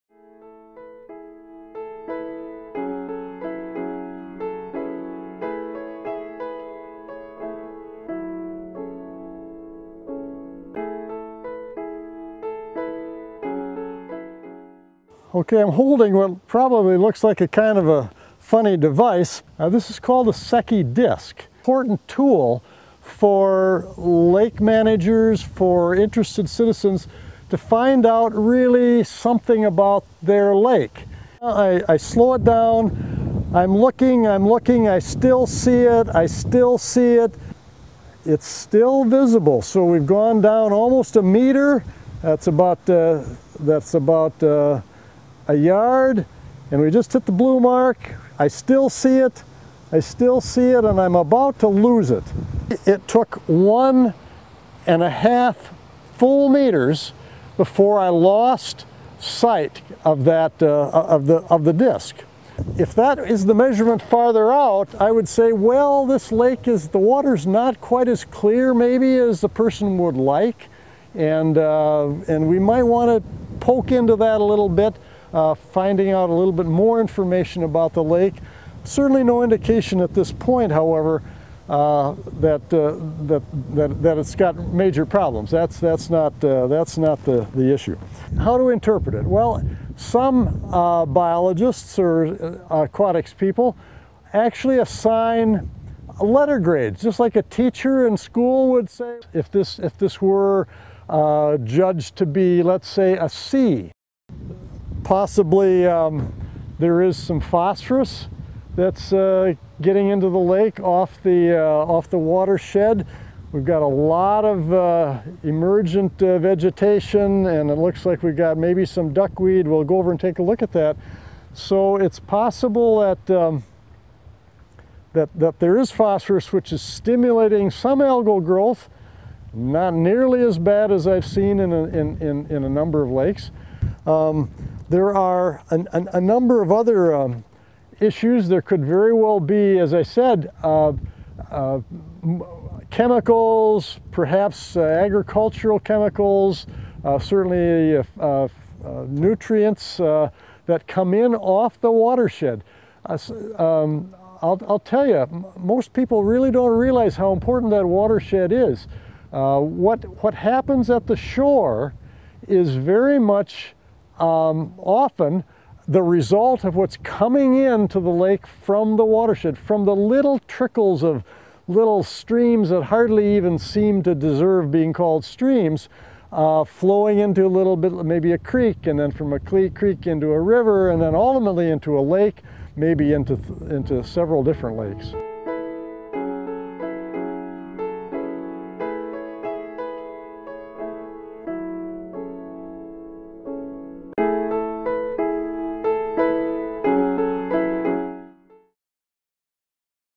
Darby Nelson Tests Lake Water Quality
Professor, Limnologist, and Author Darby Nelson describes testing a lake for clarity and the causes of green water in central Minnesota.